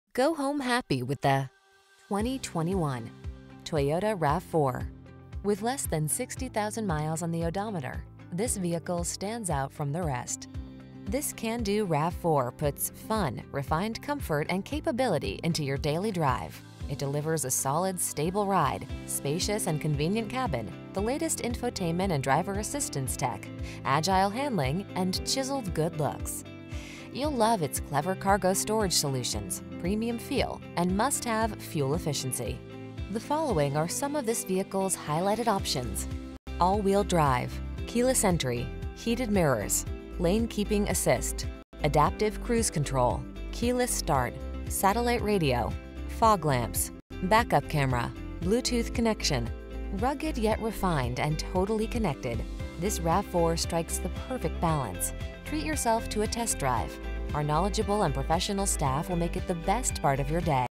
2021 Toyota RAV4 Rochester, Victor, Pittsford, Webster, Spencerport, NY VT44455
Blue Used 2021 Toyota RAV4 Adventure available in Rochester, New York at Auction Direct USA Rochester. Servicing the Victor, Pittsford, Webster, Spencerport, NY area.

New Brakes! AWD! Bluetooth, Backup Camera, Leather Interior, Power Driver Seat, Lane Assist, Remote Keyless Entry, Push Button Start, Steering Wheel Mounted Audio Controls, Touchscreen, Satellite Radio Capable, USB Port, Alloy Wheels. This vehicle has a clean, one-owner CARFAX history report and gets 33 miles per gallon on the highway!!